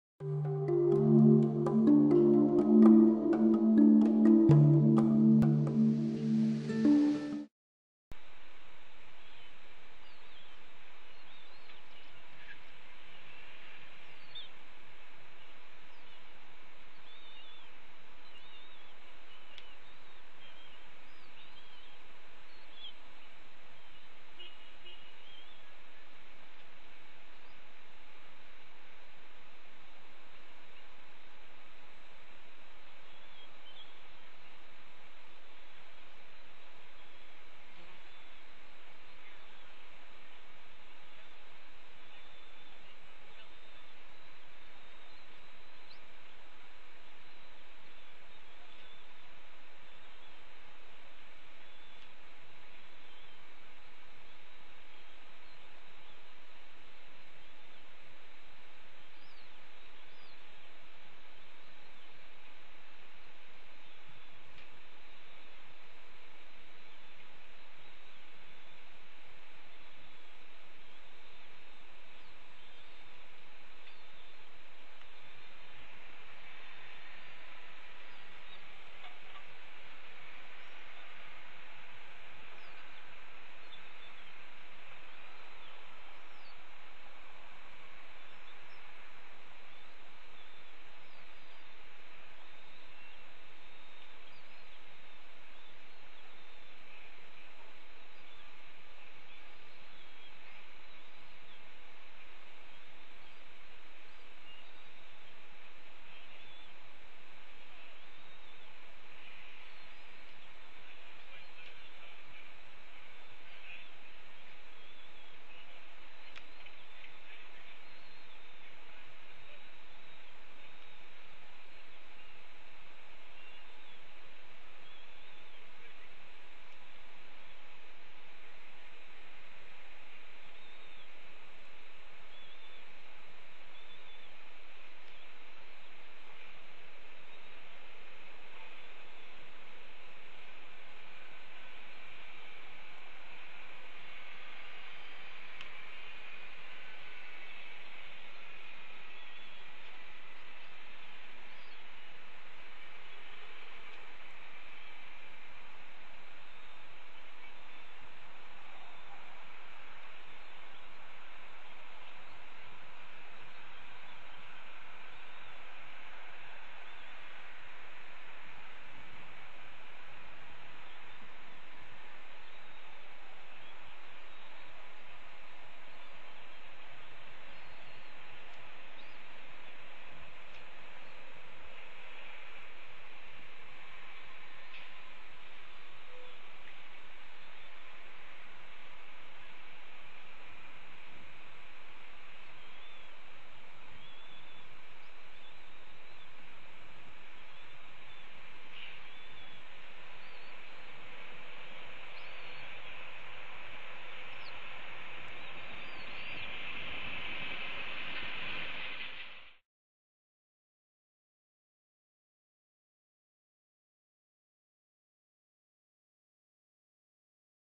Cornell RTH 'D3 Wakes Up Hungry!' 1:13 pm _5.06.13_
D3 is first in line at the squirrel lunch and is a very hungry nestling! He/She even gets a little huffy with sibling until he/she is full!